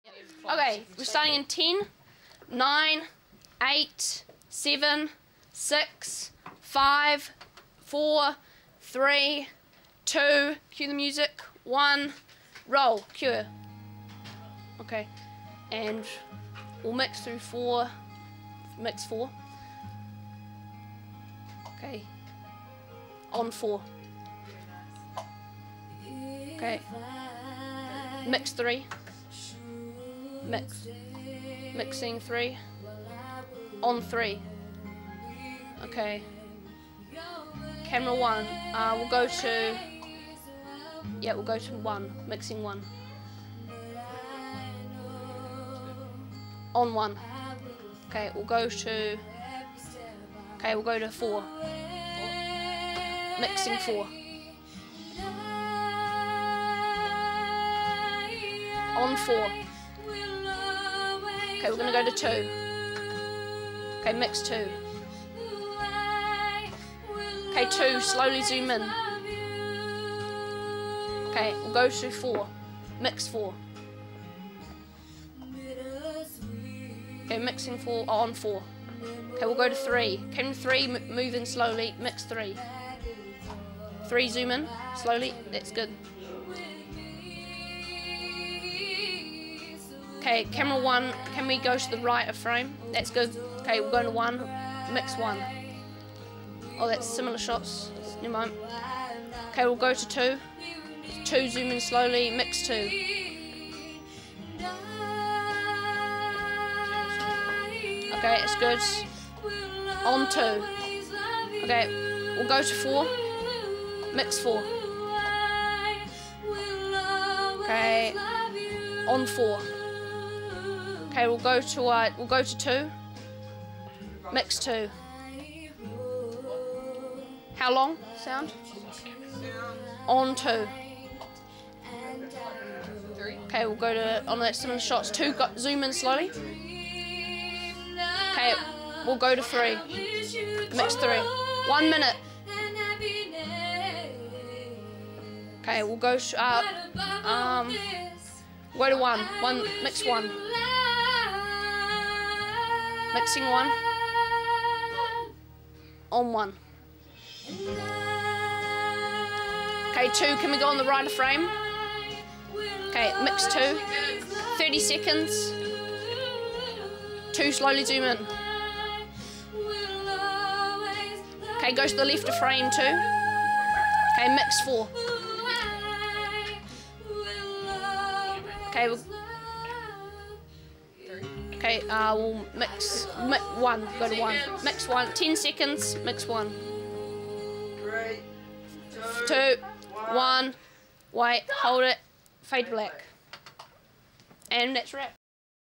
StAC - Making a tv show
Year 9 media make a tv show as part of their media course. Running a professional studio is a team effort needing concentration and discipline. Here's a St Andrew's College year 9 class producing part of a tv show - directed by Mason Gardner with Isabella Garbett performing.